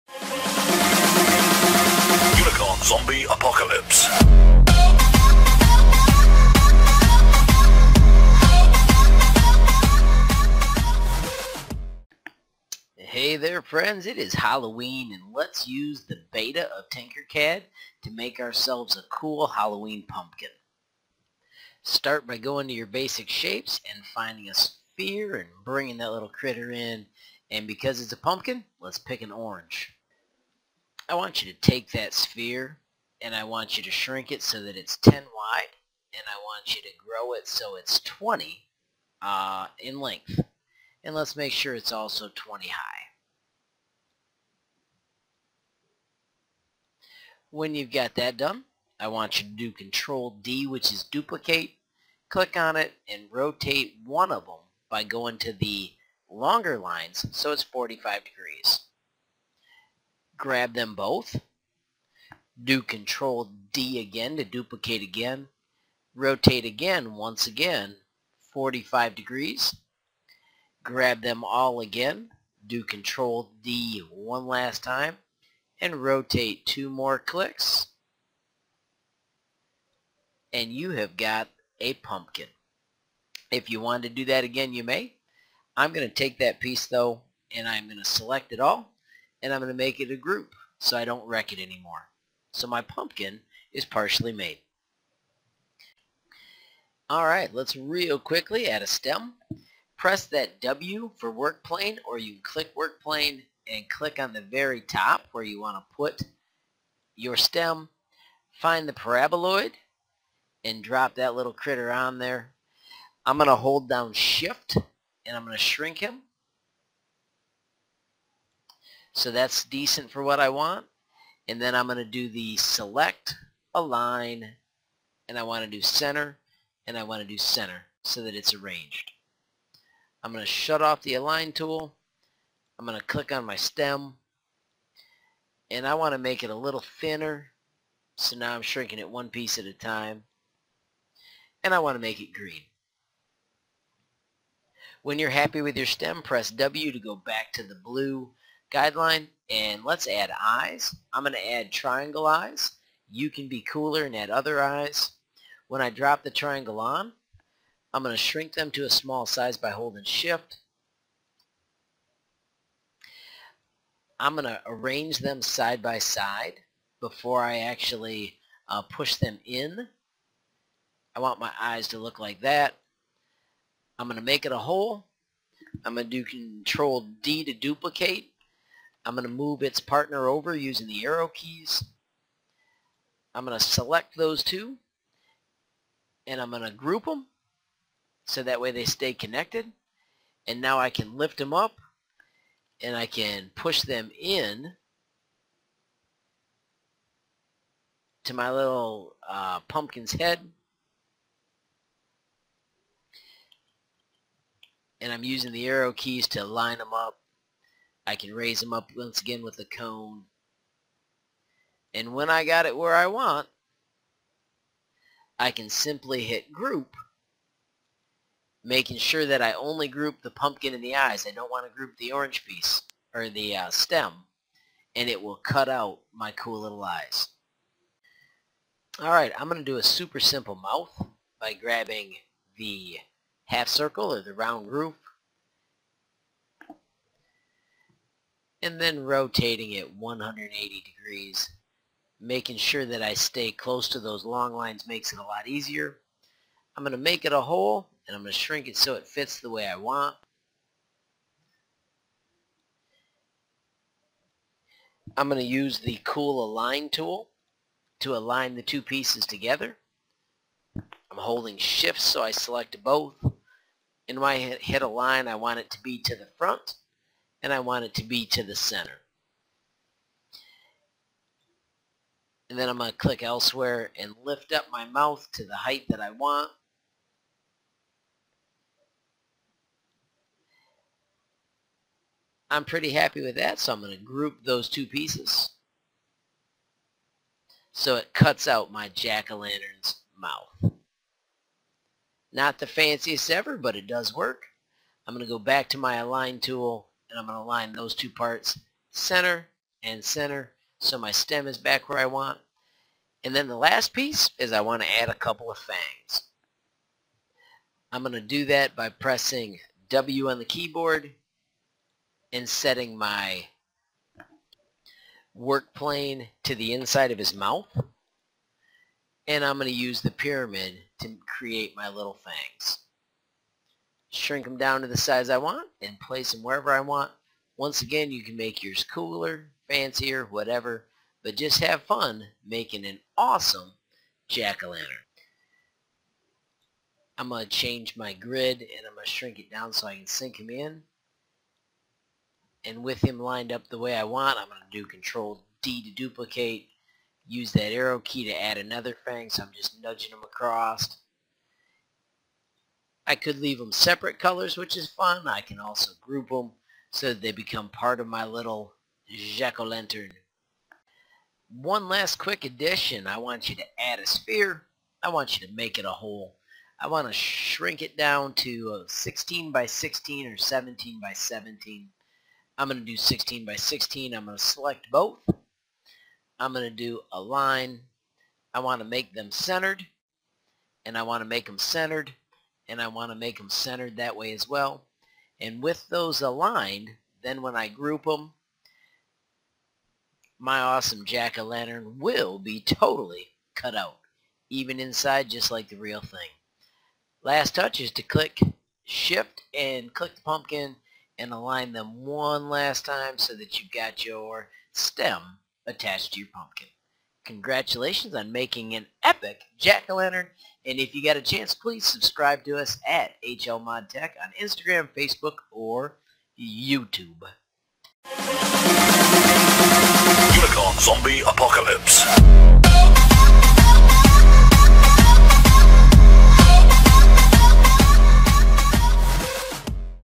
Create a Pumpkin on Tinkercad
Have a glorious day and keep Tinkering! This is a quick tutorial to get you started building a Jack O Lantern or carved pumpkin on Tinkercad